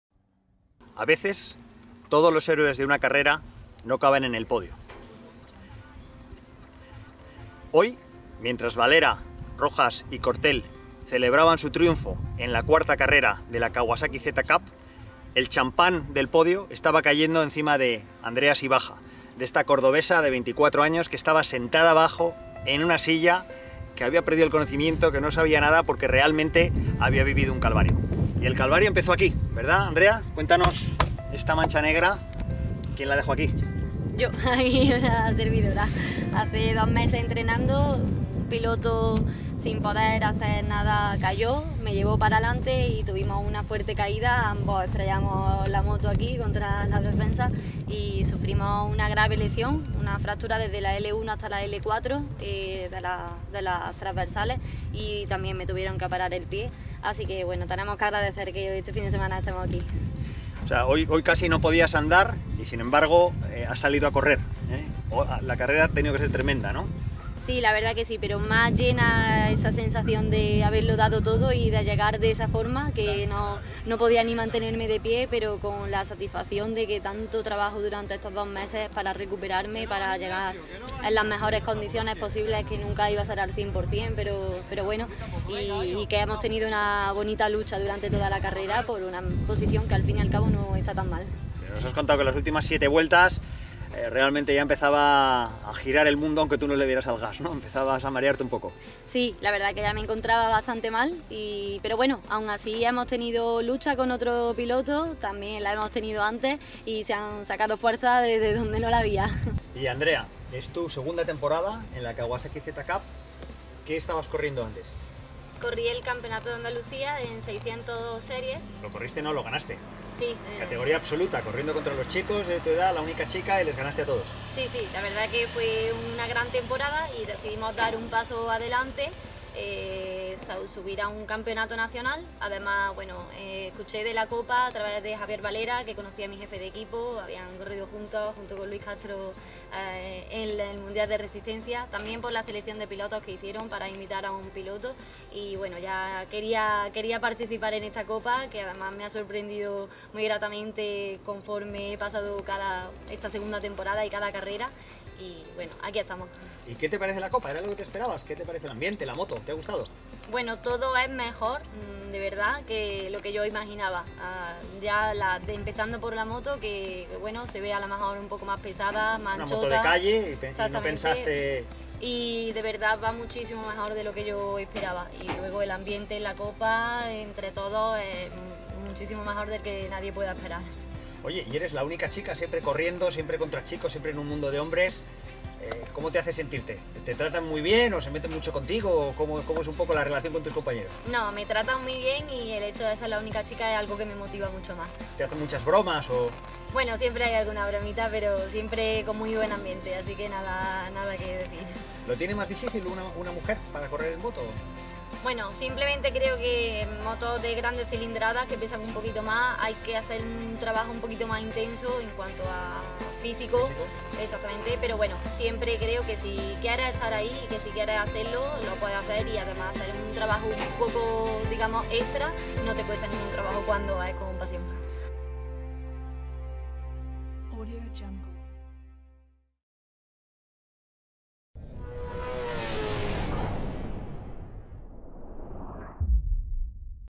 Entrevista Andrea Sibaja - Piloto Z Cup - Jerez 2016
Andrea Sibaja, la única fémina en la Kawasaki Z Cup, nos cuenta porque esta carrera de Jerez 2016 fue un calvario para ella y el sabor a victoria sin subir al podio.